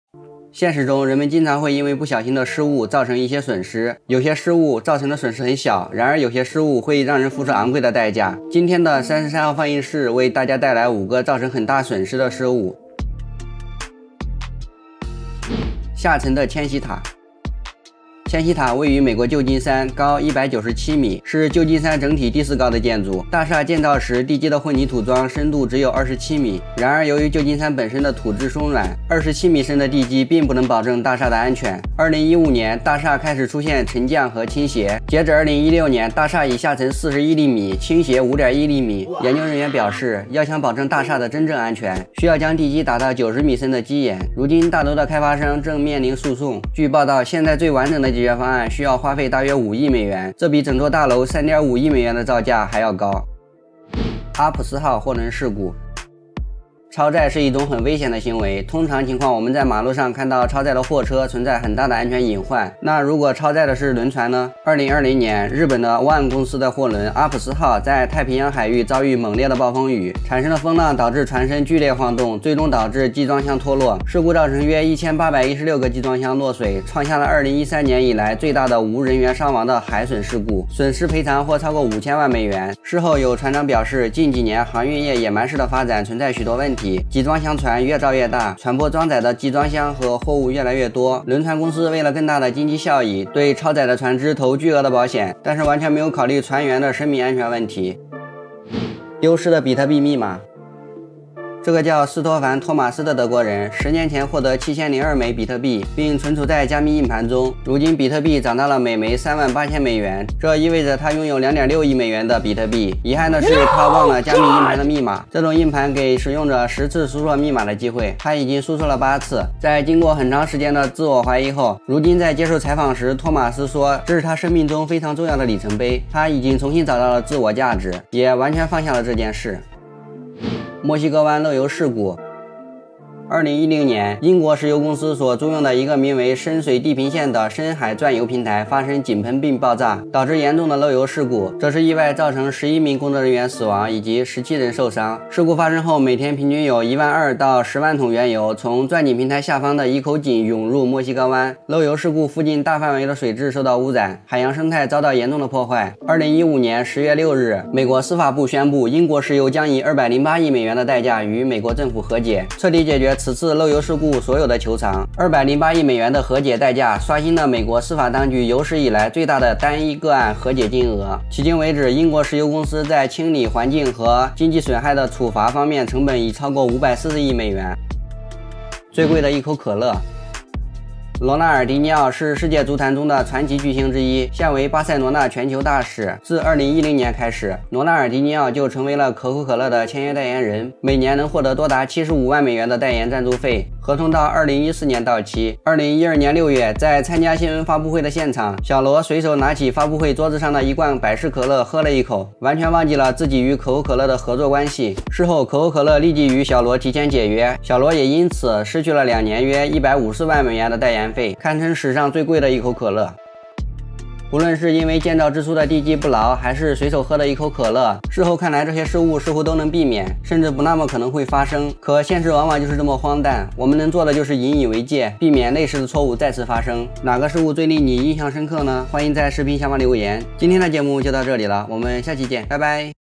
5个造成巨大损失的“失误”，只因喝了一口可乐，让他损失960万
现实中，人们经常会因为不小心的失误，造成一些损失，有些失误造成损失很小，然而有些失误会让人付出昂贵的代价。今天的33号放映室为大家带来五个造成很大损失的失误。